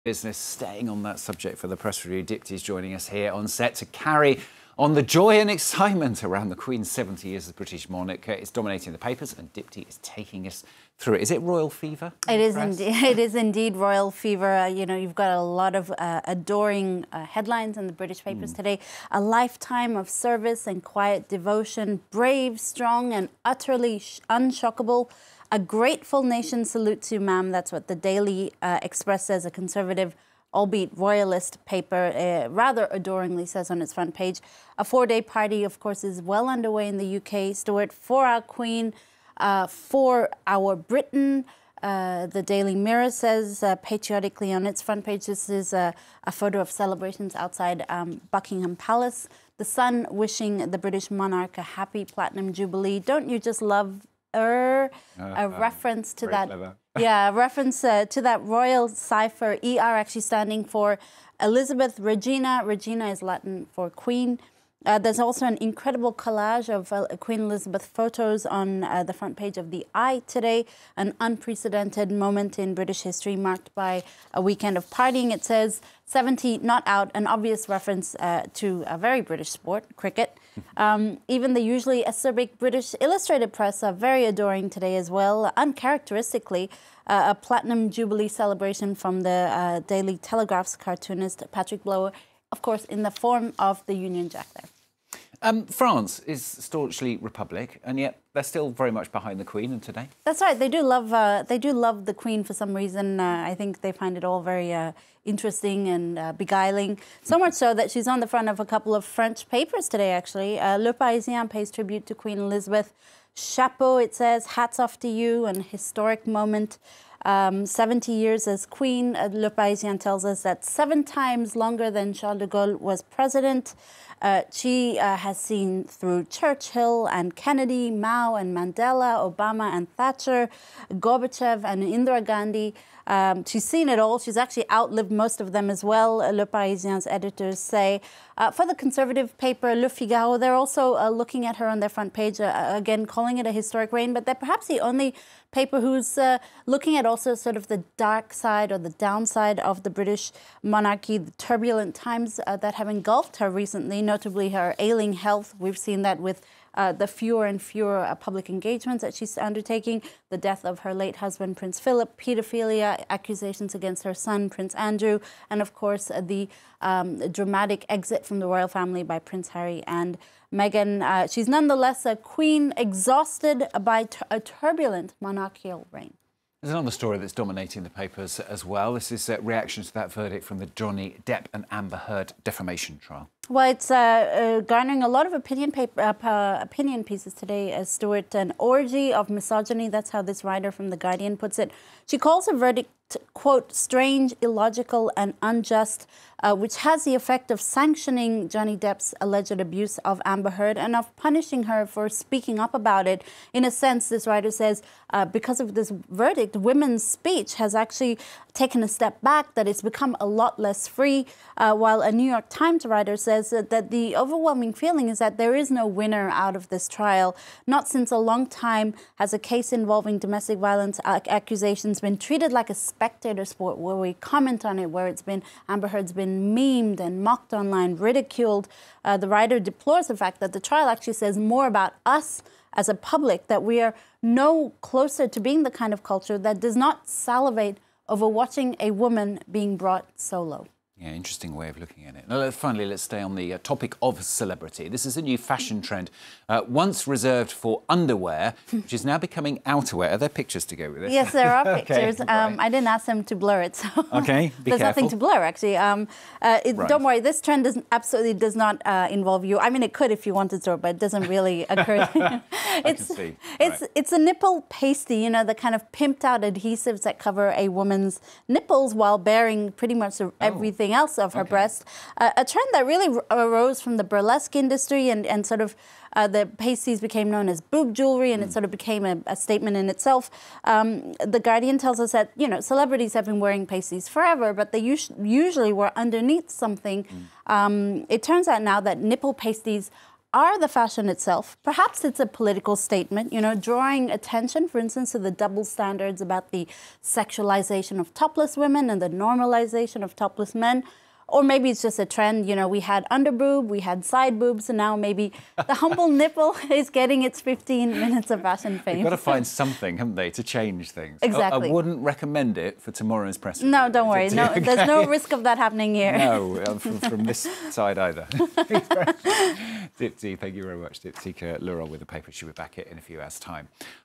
'Brave, strong and utterly unshockable': Papers pay tribute to Queen on her Platinum Jubilee
IN THE PAPERS – Thursday, June 2: It's royal fever! We look at joy and excitement in the papers as Queen Elizabeth II's Platinum Jubilee celebrations get underway. We also take a look at reactions to the verdict in Johnny Depp and Amber Heard's defamation trial. Finally, we talk about nipple pasties and how they've gone from underwear to outerwear!